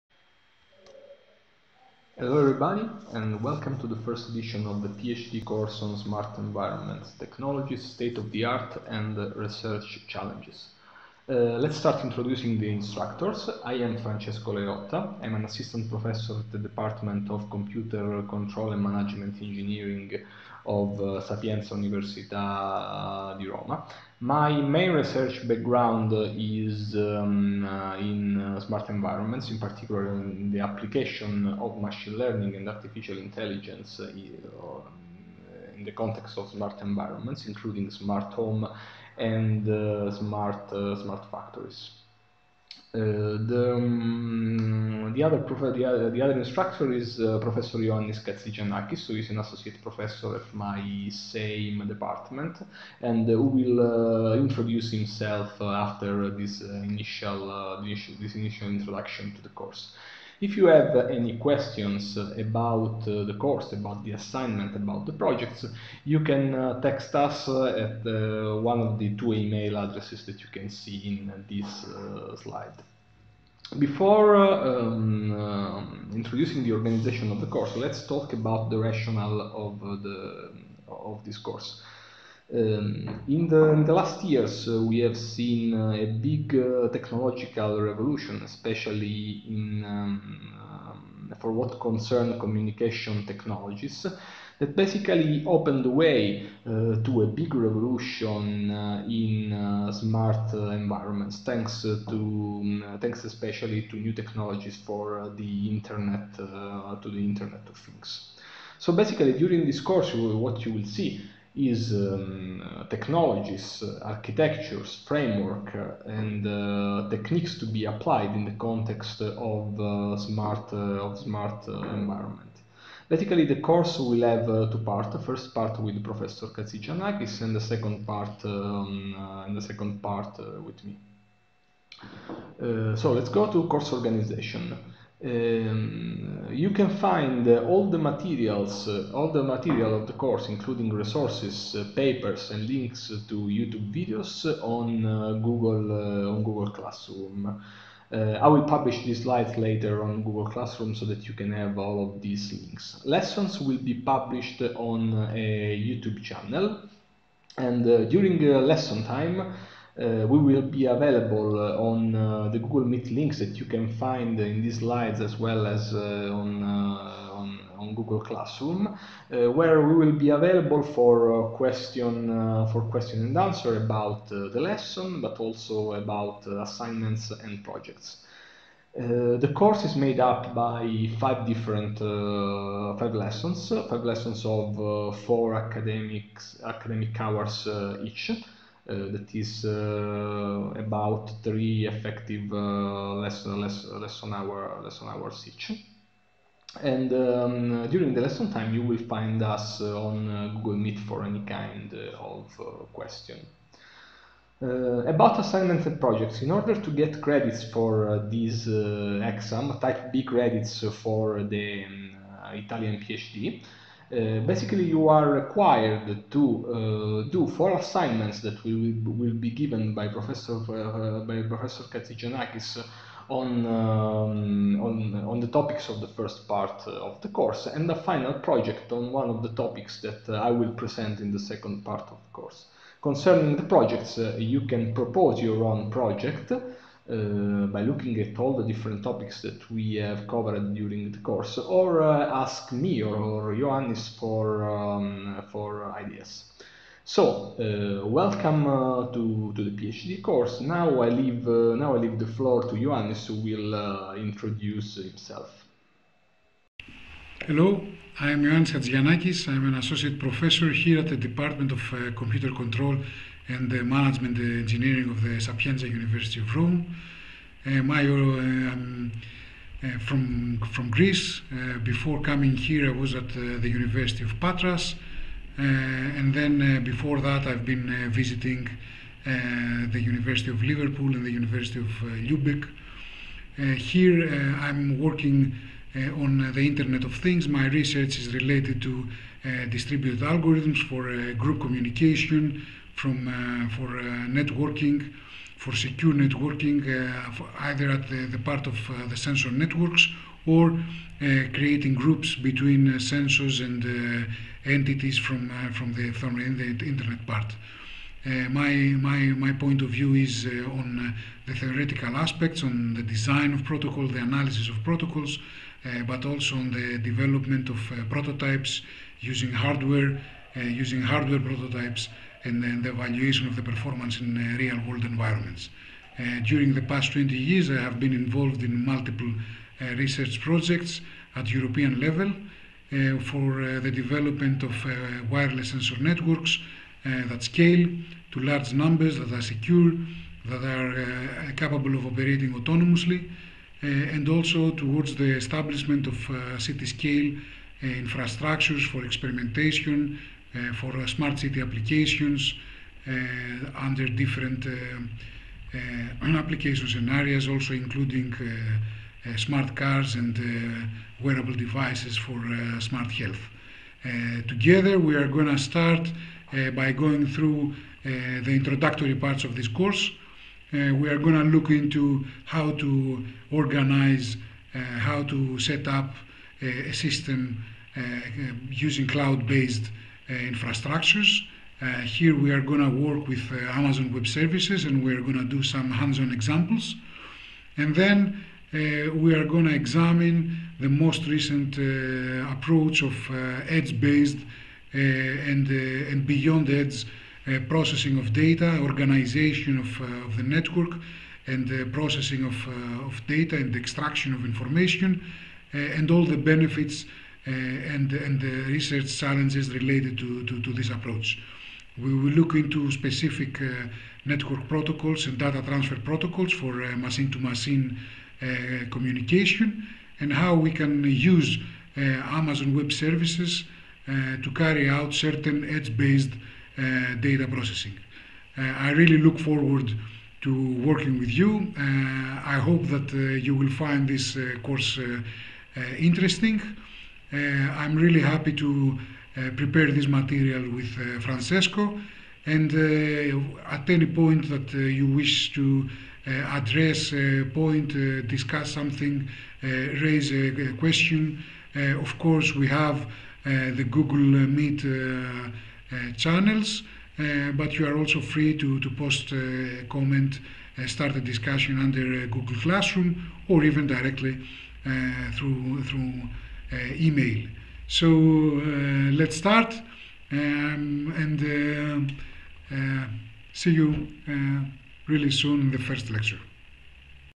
DIAG 2020 PhD Course on Smart Environments: Course Overview
Overview of PhD Course on Smart Environments carried out at the Department of Computer, Control, and Management Engineering Antonio Ruberti (DIAG) of the Sapienza University of Rome.

Lecturers: Ioannis Chatzigiannakis and Francesco Leotta